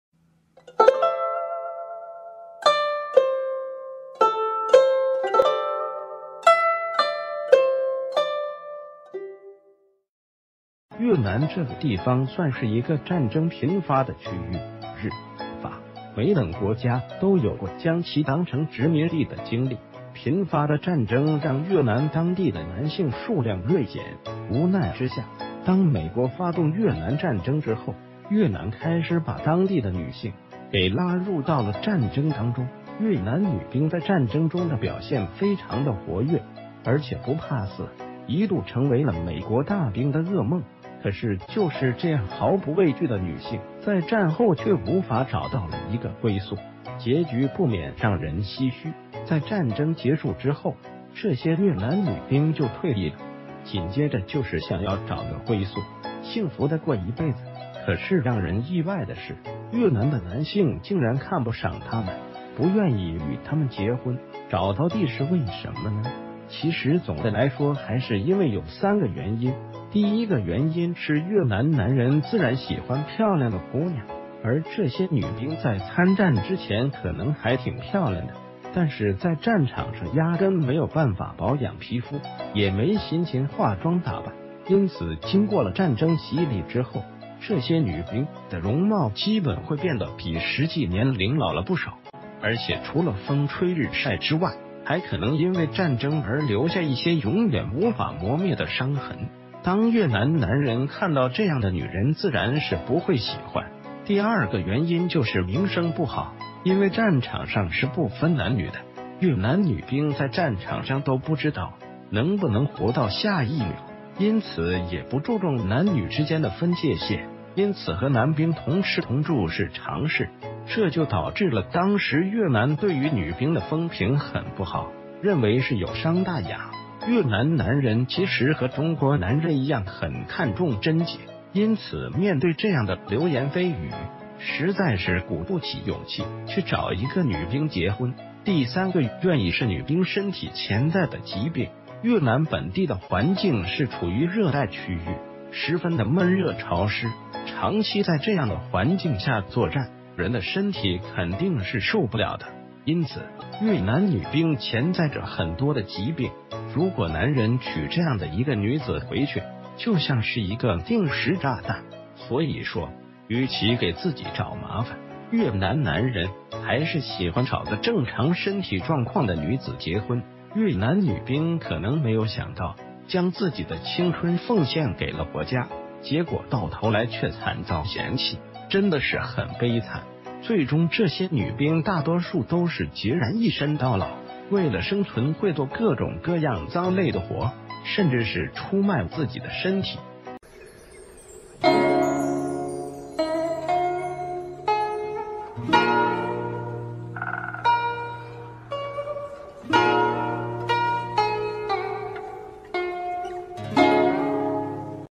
越戰結束后女兵想要找個歸宿，沒想到慘遭嫌棄，只因這三個原因
越南這個地方算是一個...